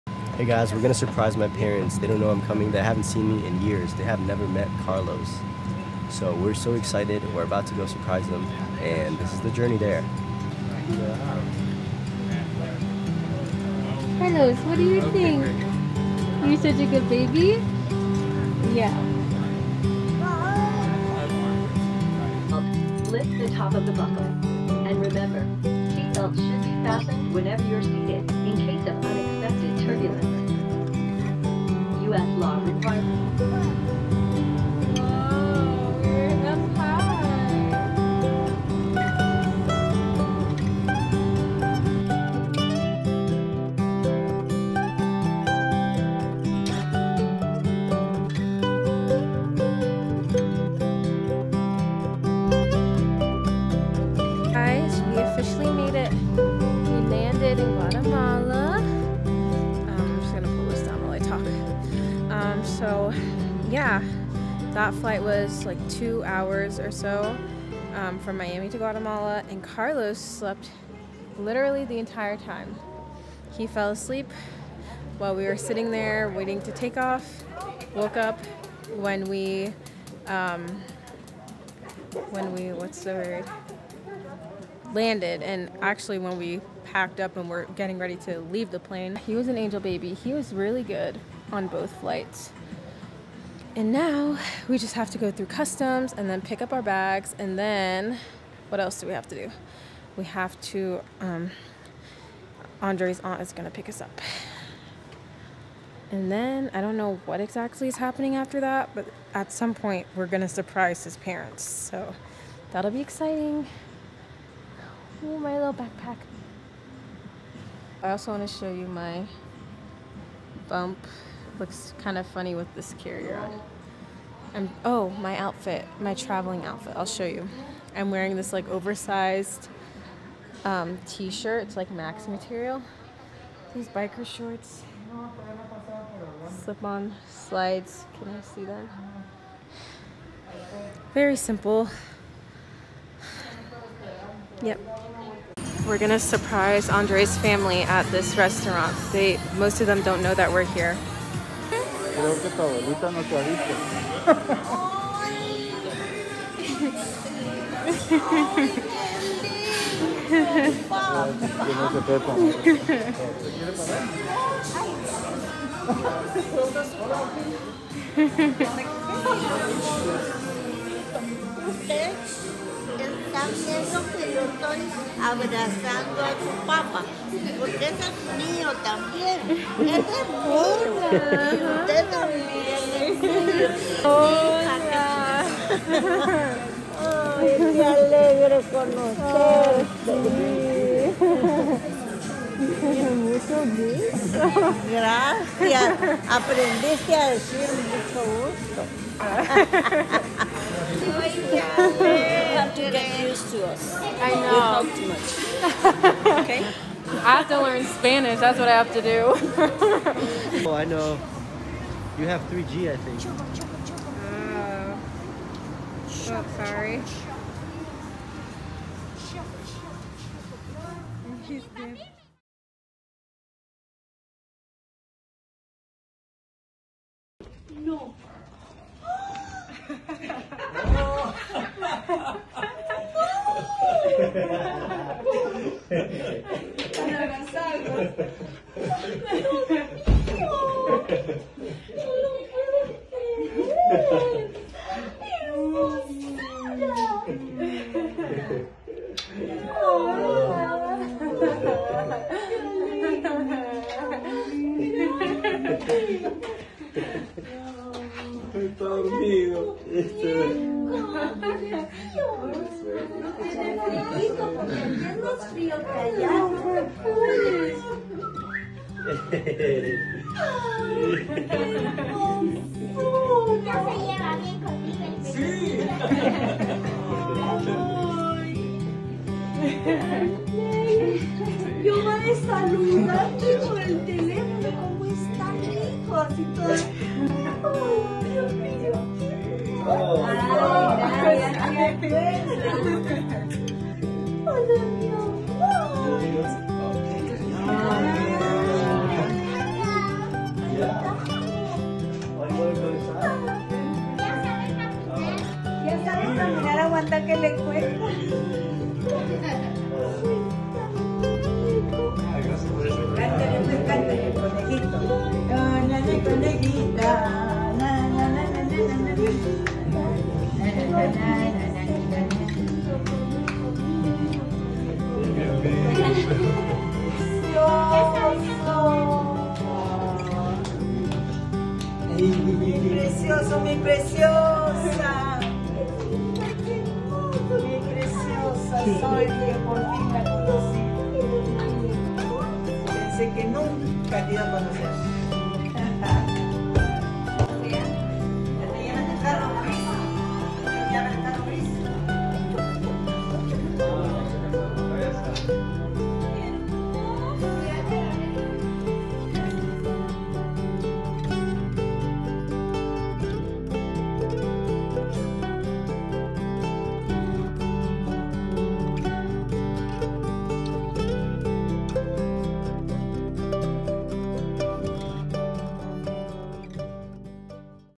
Surprising My Husband's Family in Guatemala | Baby's First Plane Ride | (Travel VLOG)
Carlos took his first plane ride as we traveled to Guatemala to surprise my husband's side of the family! I hope you enjoy!